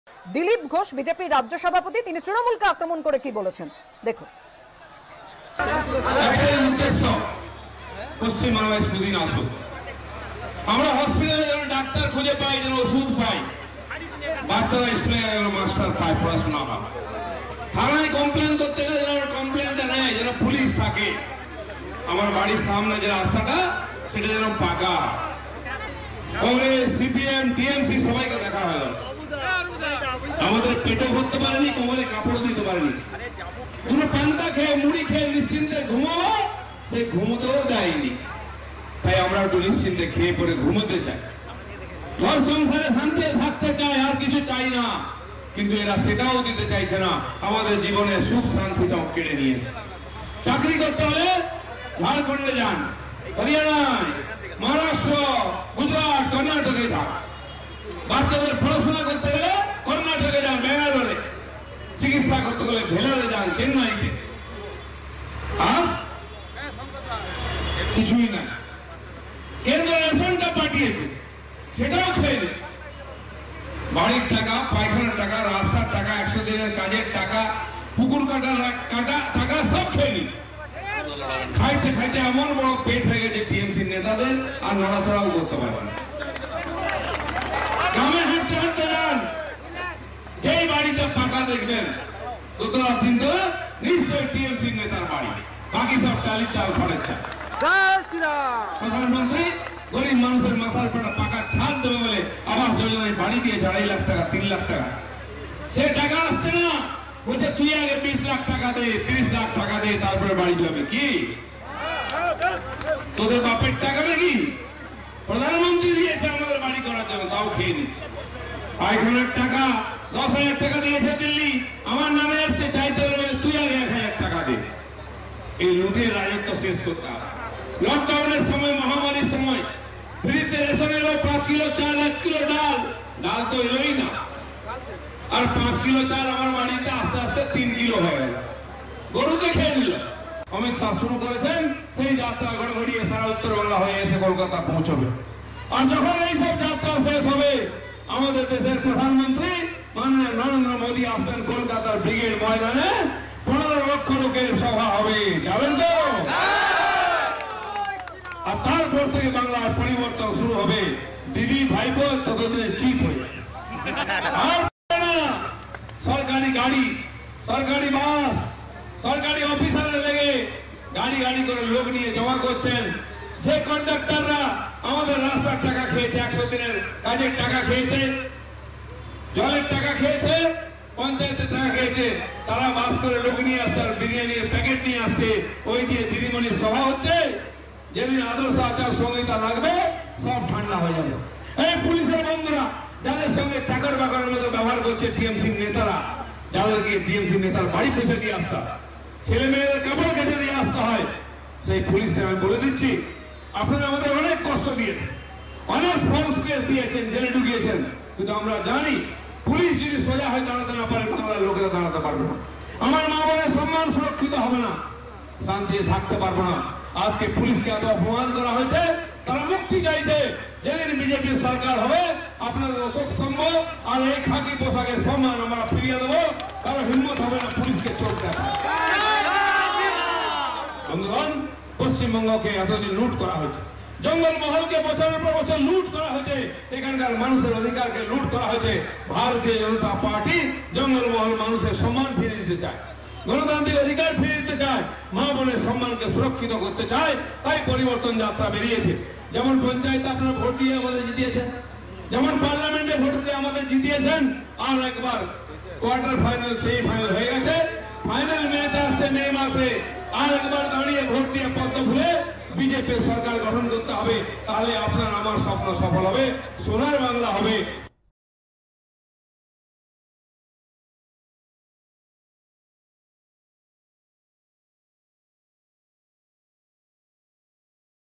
West bengal Election 2021: ব্রিগেডে ১৫ লক্ষ লোকের সভা হবে, তারপরেই বাংলায় পরিবর্তন, দাবি দিলীপ ঘোষের
মেদিনীপুর শহরে পরিবর্তন যাত্রার সূচনা করলেন দিলীপ ঘোষ। কেরানিচটি, ভাদুতলা, শালবনি, চন্দ্রকোণা রোড, গড়বেতা, রাসকুণ্ডু হয়ে বিজেপির রথ পৌঁছবে চন্দ্রকোণা শহরে। এরই মধ্যে রথ থামিয়ে পথসভা করেন বিজেপির রাজ্য সভাপতি তথা মেদিনীপুরের সাংসদ দিলীপ ঘোষ। চন্দ্রকোণা রোডের সভা থেকে তৃণমূলকে আক্রমণ করে তিনি বলেন, ‘কংগ্রেস, সিপিএম, তৃণমূল-সবাইকে দেখা হয়ে গেল। আমরা খেয়ে-পরে নিশ্চিন্তে ঘুমোতে চাই। কিন্তু এরা আমাদের জীবন থেকে সুখ-শান্তিটুকুও কেড়ে নিয়েছে। বাংলায় চিকিৎসা, পড়াশোনা, চাকরির কোনও সুযোগ নেই। কেন্দ্র যে রেশন পাঠিয়েছে, সেটাও খেয়ে নিয়েছে তৃণমূলের নেতারা। ইমিত শাহ যে যাত্রা শুরু করেছেন, সেই যাত্রা এসে কলকাতায় পৌঁছবে। তখন প্রধানমন্ত্রী নরেন্দ্র মোদি এসে ব্রিগেডে সভা করবেন। ১৫ লক্ষ লোকের সভা হবে। তারপরেই বাংলায় পরিবর্তন শুরু হবে।’